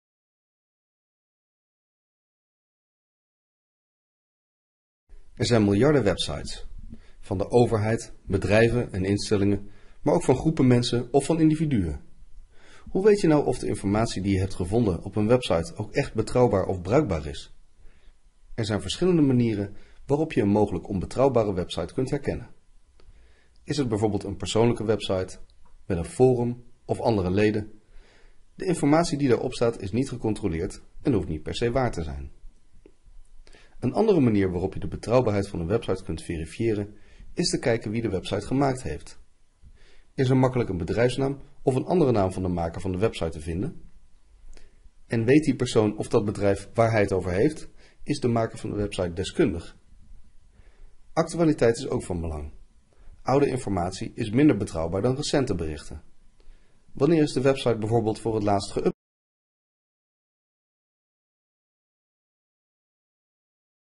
Websites beoordelen op betrouwbaarheid
Wil je leren hoe je websites op betrouwbaarheid kunt beoordelen? Bekijk dan deze instructiefilm van SPONS Learning Company. Meer leren? Bezoek en ontdek onze online modules over werken en lesgeven met nieuwe media!